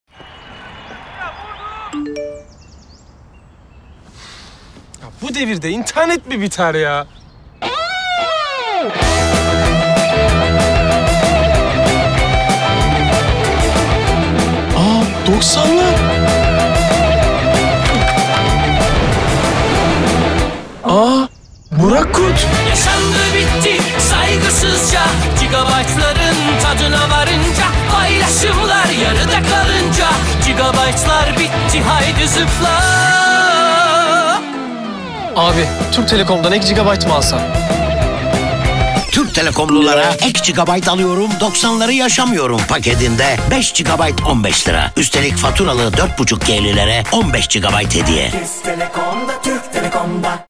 Türk Telekom’dan Ek GB alıyorum, 90’ları Yaşamıyorum Paketi Reklam Filmi
Türk Telekom’lulara “Ek GB Alıyorum 90’ları yaşamıyorum paketi” 5GB 15 TL, üstelik faturalı 4.5G’lilere 15 GB hediye. Ayrıntılar